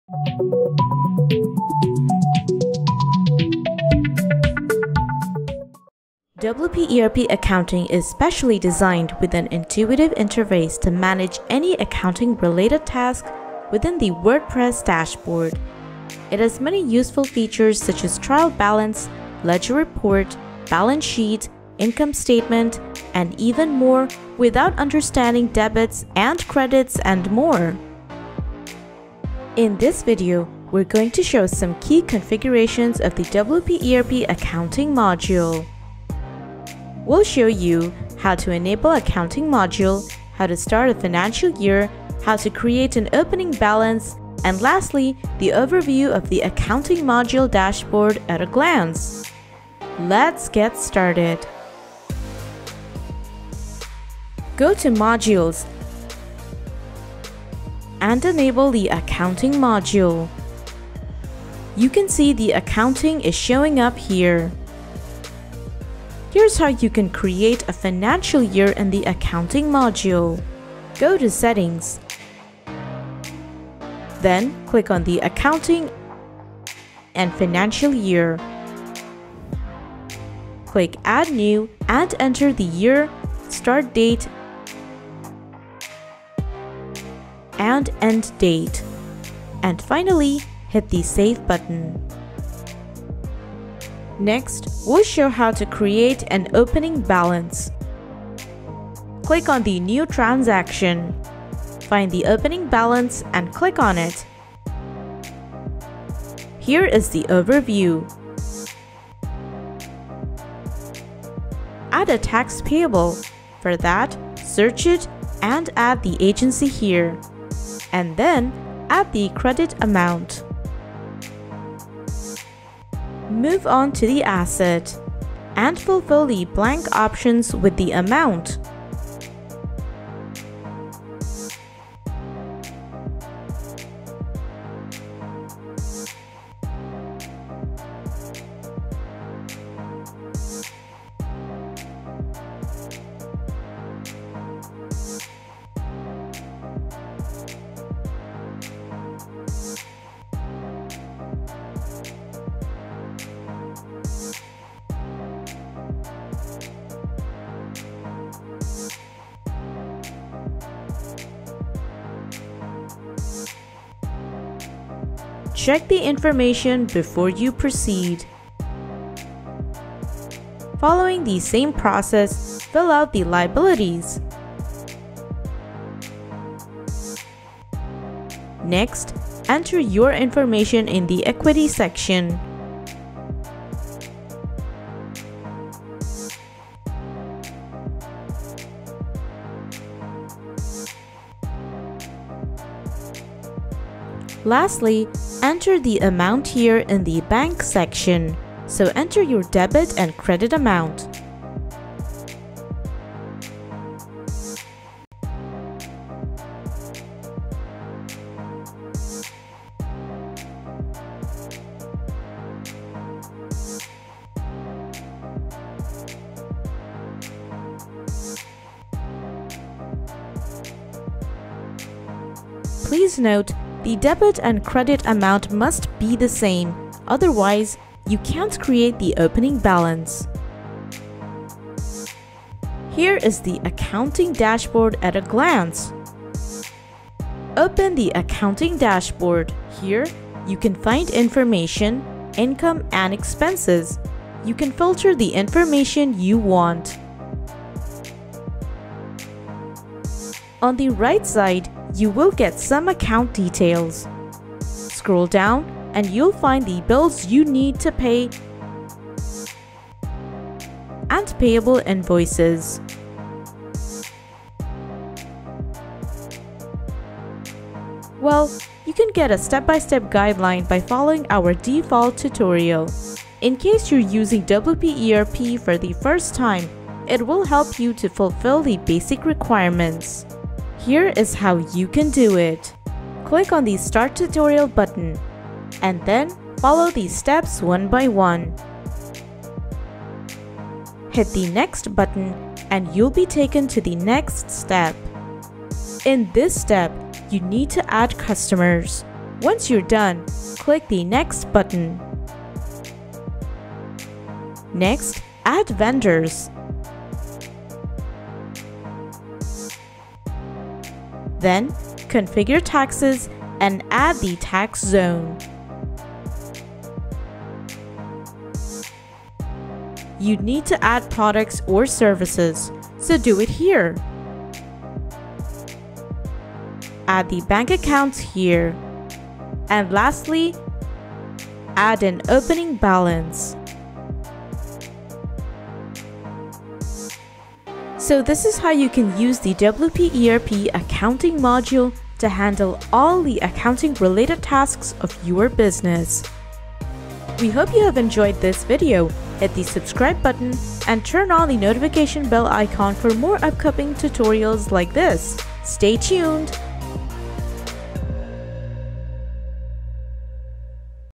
WP ERP Accounting Module At a Glance [How to Install & Activate]
WP ERP Accounting was made with an easy-to-use interface so that you can handle any accounting task from the WordPress dashboard. It has so many useful features, like a trial balance, ledger report, balance sheets, income statements, and more, that you can use it even if you don't know how debits and credits work.

Let’s see how to use the WP ERP Accounting module on your WordPress site:

Time-Stamp:
Introduction: 0:00
Talking about ERP accounting module: 0:05
Checklist: 0:42
Enabling ERP Accounting module: 0:49
Creating financial year: 1:06
Creating an opening balance: 1:34
Accounting module overview: 3:52
Step-by-step guide on Accounting tutorial: 4:42

Get connected through our social channels